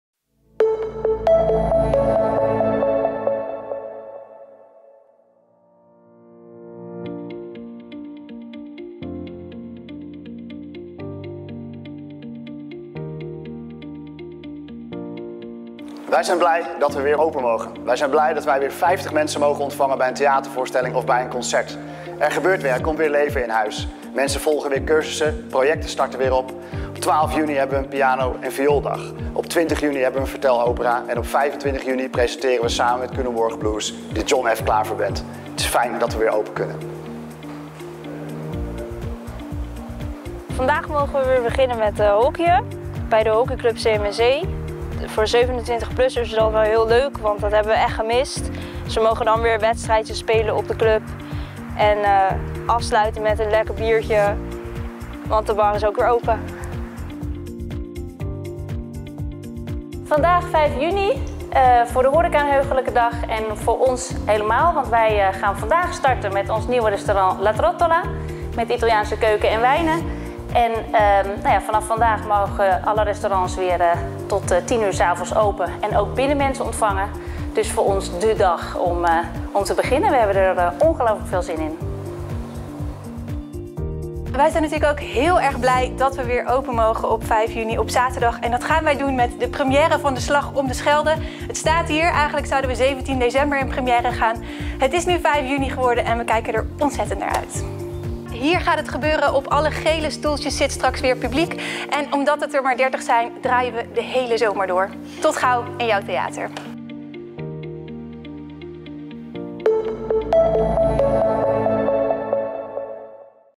Versoepelingen coronamaatregelen 5 juni
Het aantal patiënten op de IC loopt terug net zoals het aantal besmettingen.  Steeds meer mensen zijn gevaccineerd en dat is een goede reden voor  versoepeling van de genomen maatregelen.
Zo mogen we thuis vier personen ontvangen in plaats van twee, culturele instellingen zoals musea, biscopen en theaters mogen weer open.  Restaurants en cafe's mogen  binnen weer gasten ontvangen.